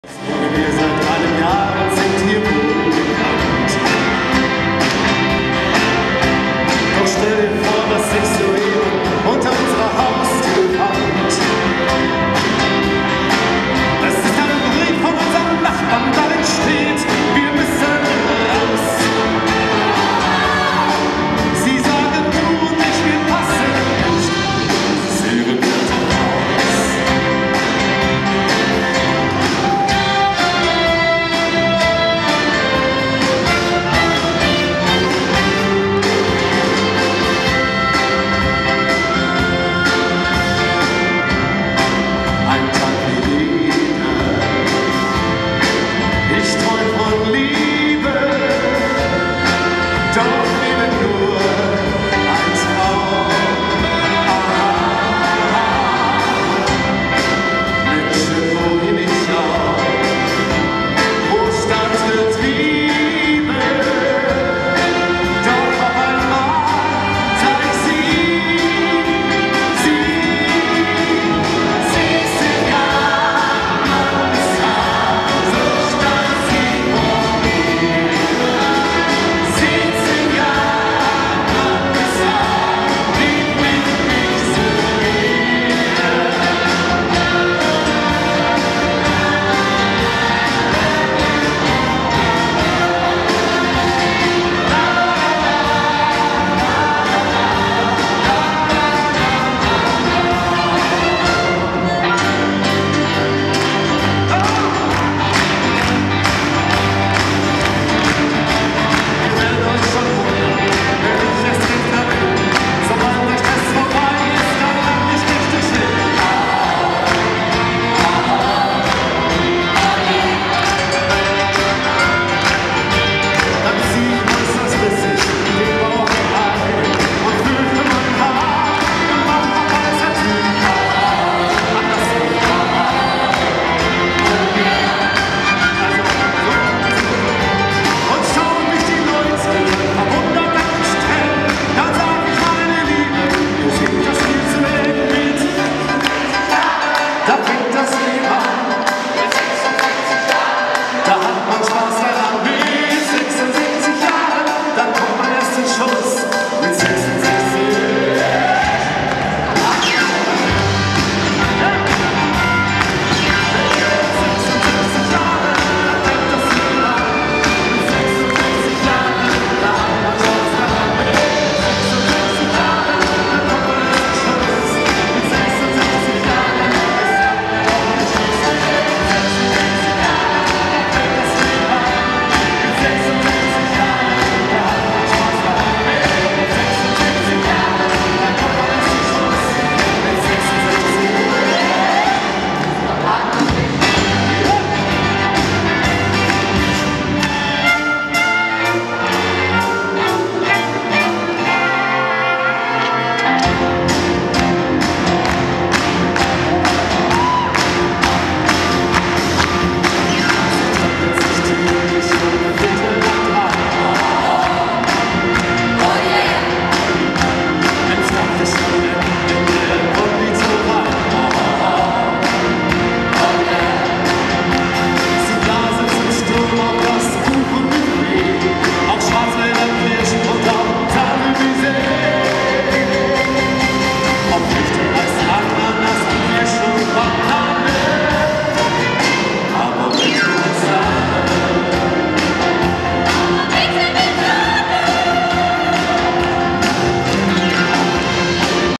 Rockorchester Ruhrgebeat Glockenrock: Udo Jürgens Medley (Livemitschnitt)
Die Aufnahme entstand während der Veranstaltung: Glockenrock des Rockorchester Ruhrgebeat am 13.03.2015 in der Erlöserkirche Essen.